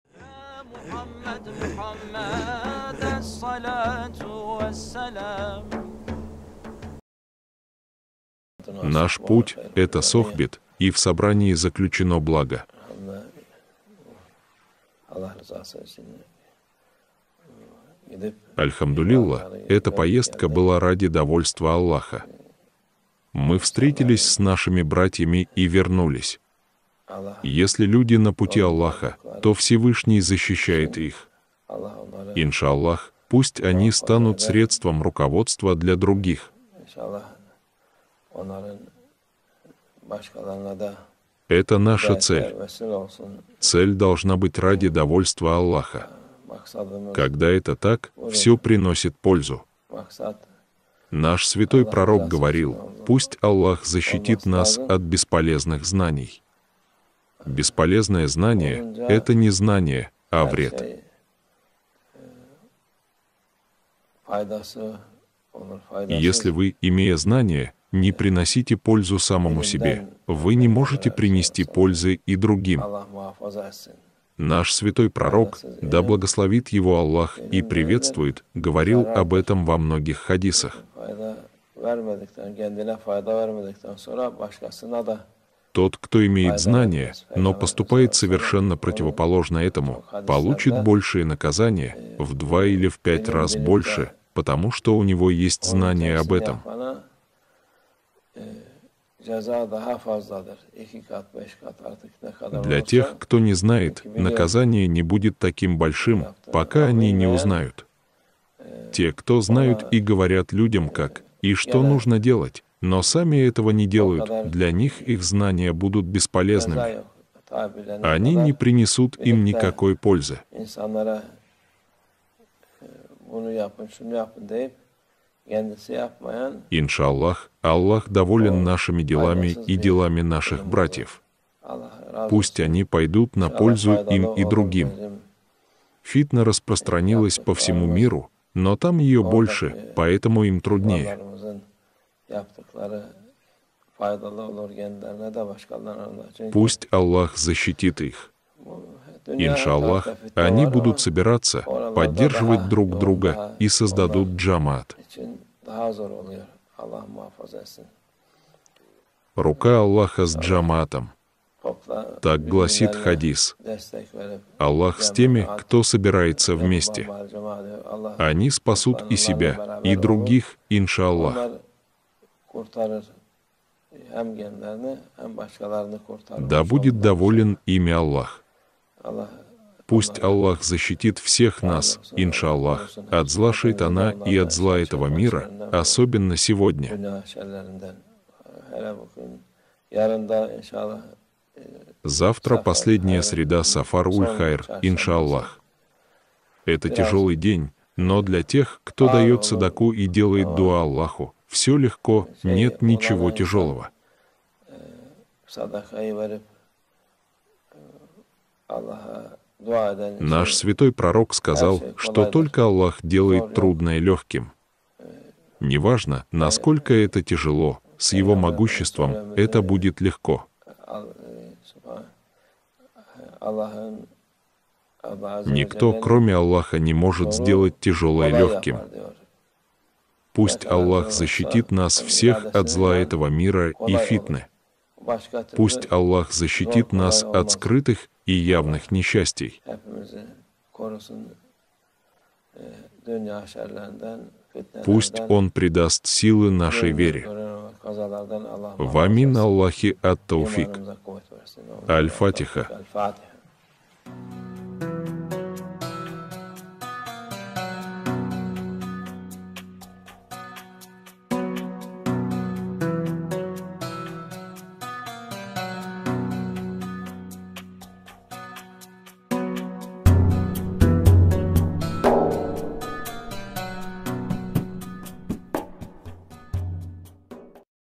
Шейх Мехмет Эфенди. Рука Аллахаﷻ - с Джама'атом
Наш Святой Пророкﷺ говорил: “Пусть Аллахﷻ защитит нас от бесполезных знаний”. Бесполезное знание - это не знание, а вред. Если вы, имея знания, не приносите пользу самому себе, вы не можете принести пользы и другим.